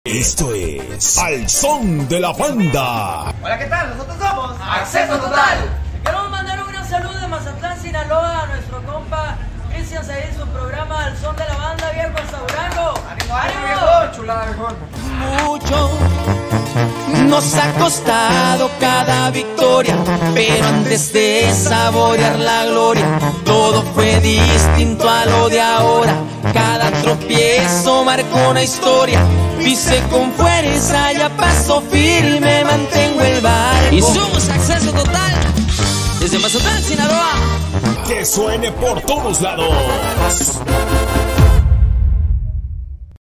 Saludos de Acceso Total para Al Son de la Banda
Nuestros amigos de Acceso Total, desde la Perla del Pacífico nos mandan un saludo, va uno de vuelta y los esperamos pronto ¡Y que suene Acceso Total, que detallazo!